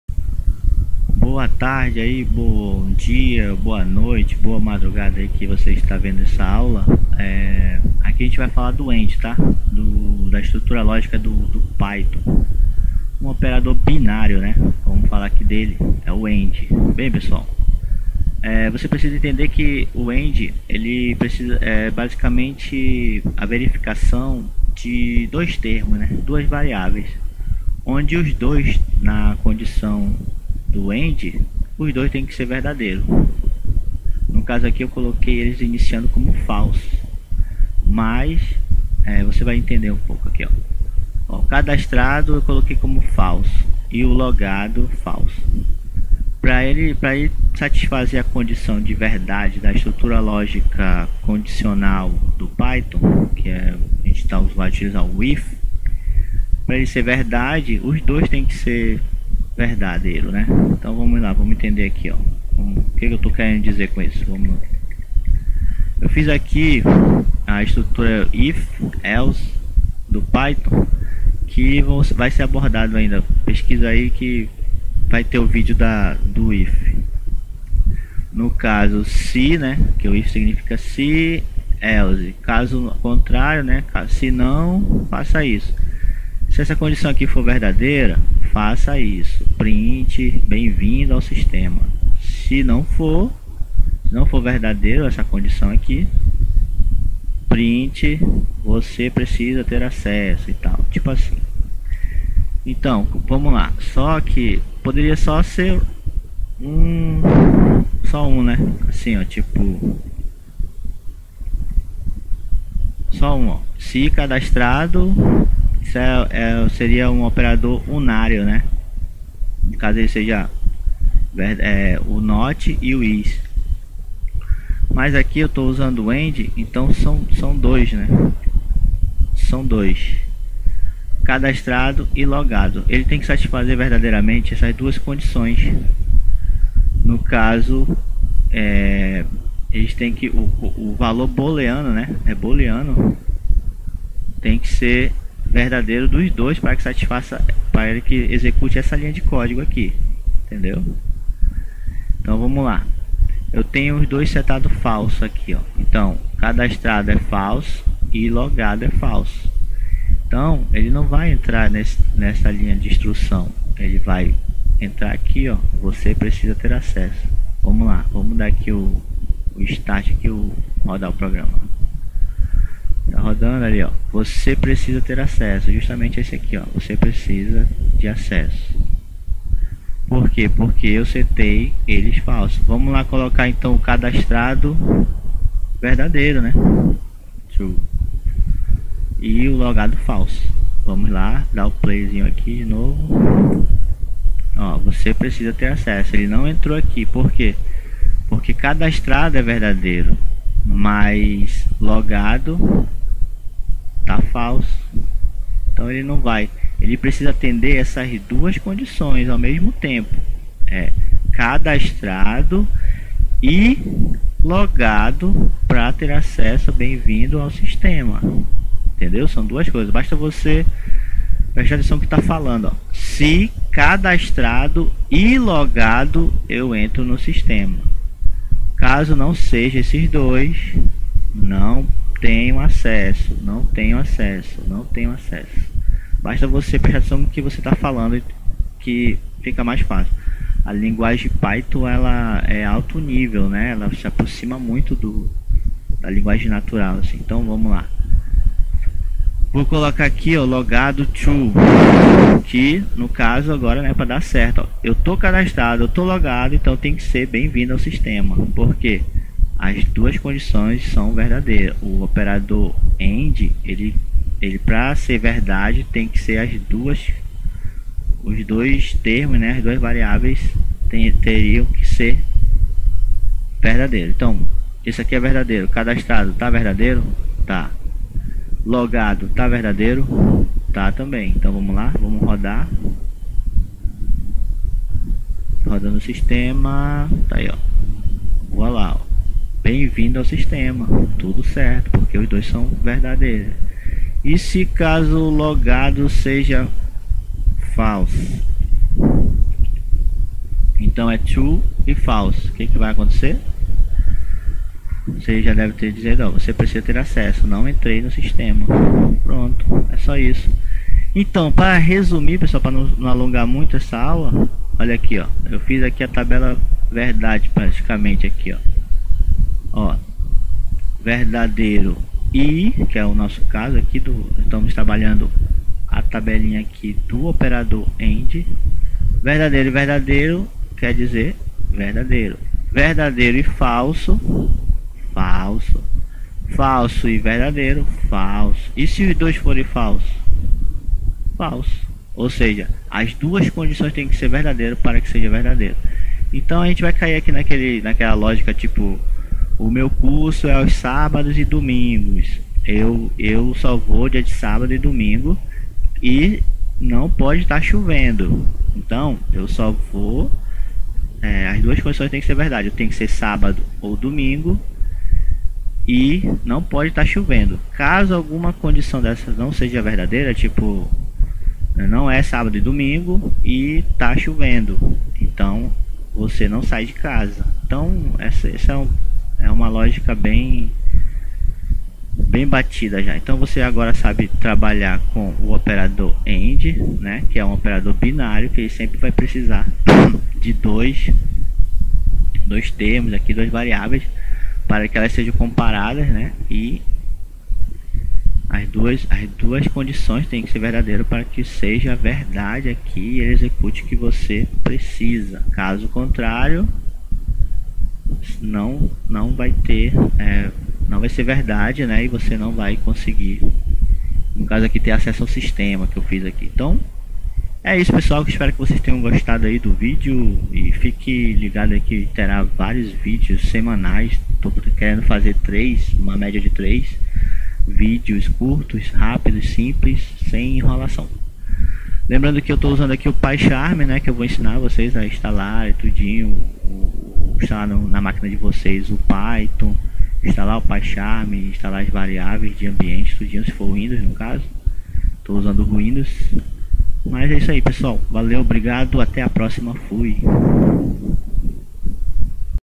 PYTHON PROGRAMAÇÃO - OPERADOR LÓGICO AND.
OPERADORES LÓGICOS
Programação python
Python programação

Seja programador Python em 2021.

Neste video vamos aprender o funcionamento do operador lógico AND da linguagem Python.

Resumo da lógica do operador AND
True   AND True   = True
True   AND False = False
False AND True   = False
False AND False = False

Programação Python.
Curso de python.